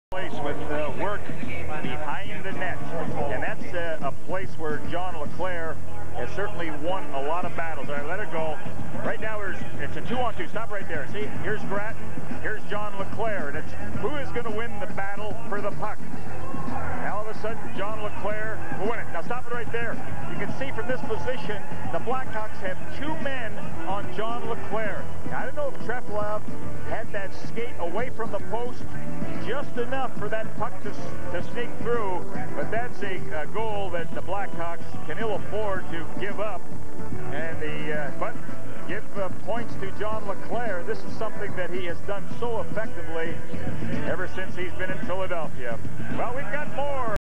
John LeClair goal (Philadelphia Flyers vs Chicago Blackhawks 12/12/1997)
John LeClair's 24th goal of the 1997-98 Philadelphia Flyers season, beating Chicago Blackhawks goalie Andrei Trefilov to tie the game at one in the first period. The December 12, 1997 game took place at United Center in Chicago. The Flyers would eventually be victorious, 3-2.

The clip comes from Gary Dornhoefer's intermission analysis on Comcast SportsNet. Chris Gratton got the assist on LeClair's goal.